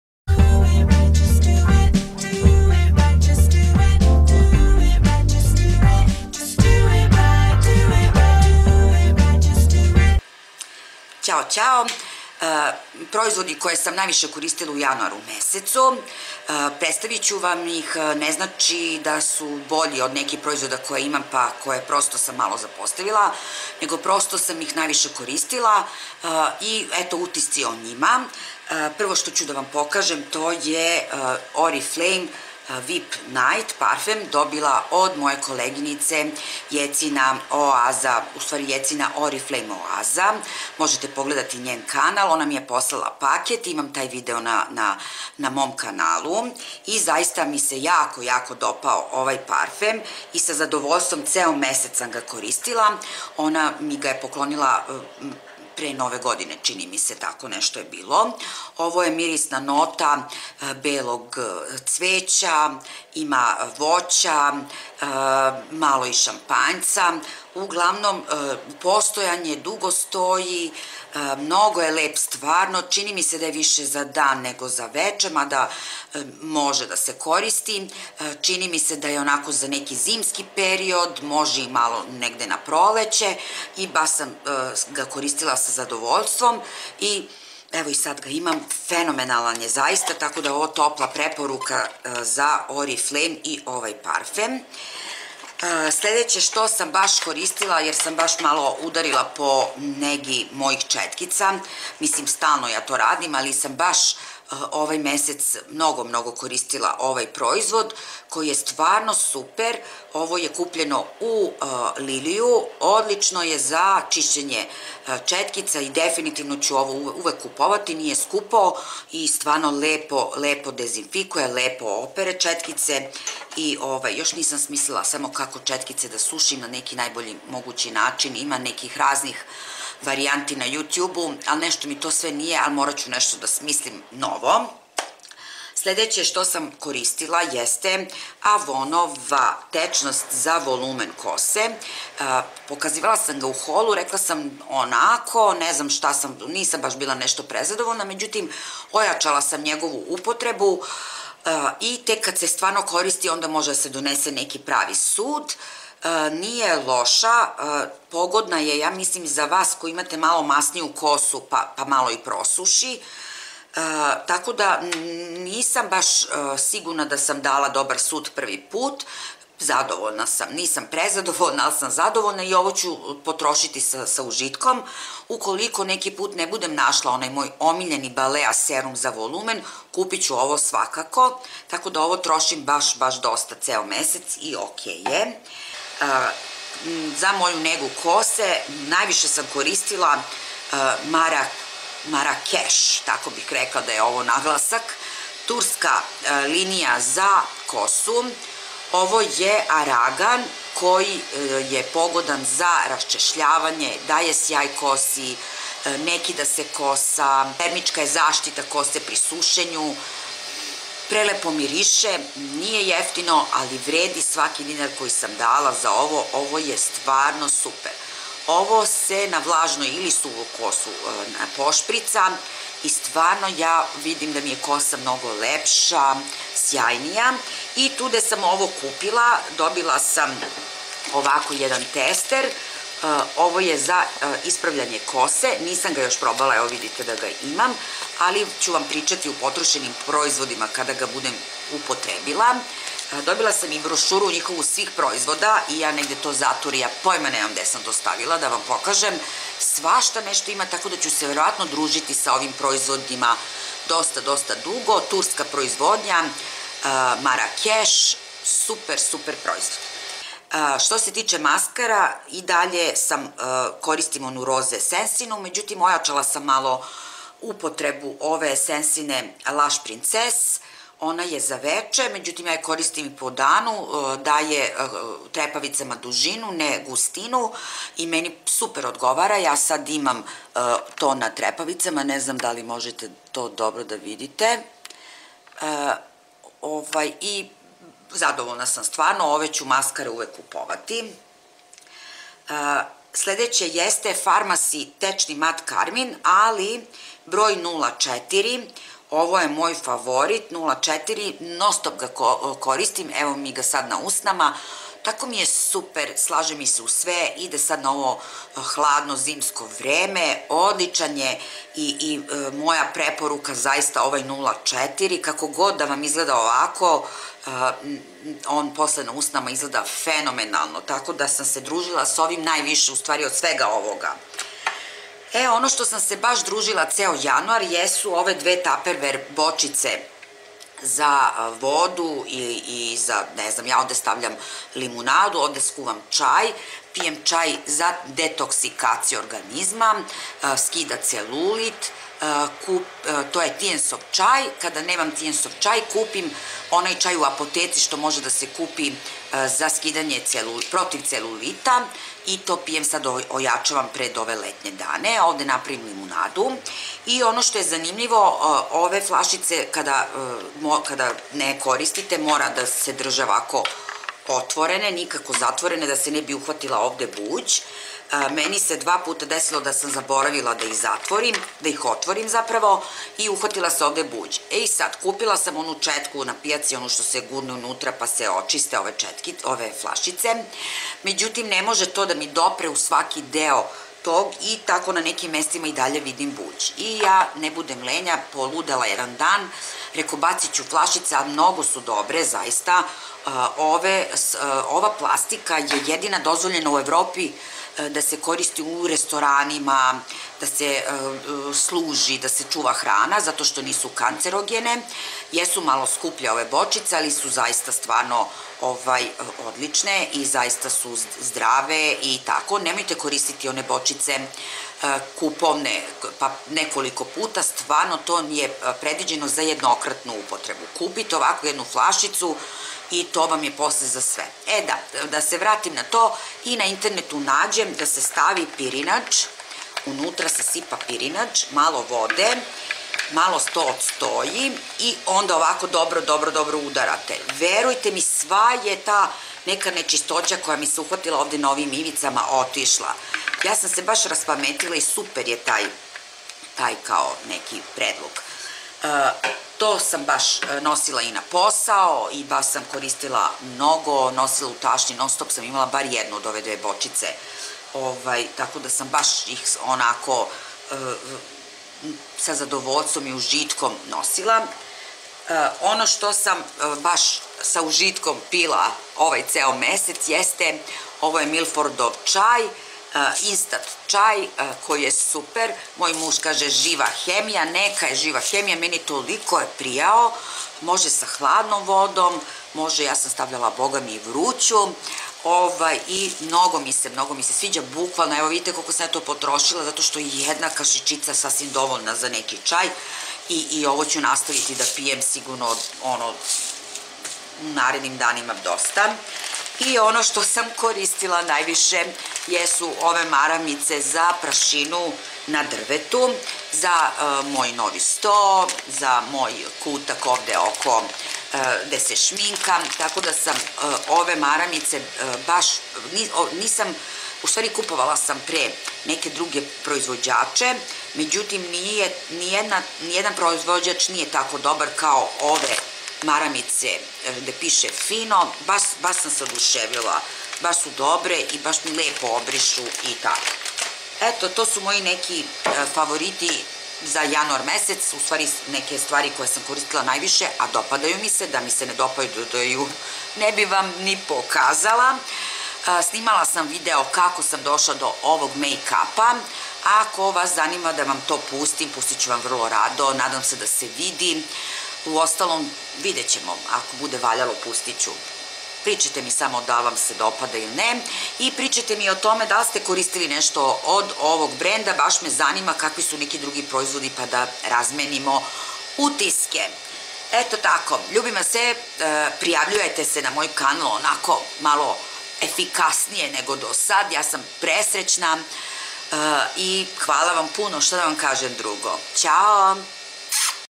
Favoriti januara 💟 January favorites 2017. #9
V A Ž N O !❤Marrakesh je profesionalna linija proizvoda za kosu iz SAD-a a ne iz Turske... izvinjavam se. Napravljena od prirodnih sastojaka. Marrakesh proizvodi sadrže arganovo ulje iz Maroka i ulje konoplje koji kosu hrane i pružaju joj prirodnu zaštitu. Proizvodi Iz linije Marrakesh ne sadrže parabene, sulfate, propilen glikole ni umetna bojila.
Hvala sto me gledate...
❤prijavite se...♔ℱℴℓℓℴω♔❤
FB STRANICA- I JA MOGU BITI LEPA
A BUDI PRIJATELJ SA- BUDI LEPA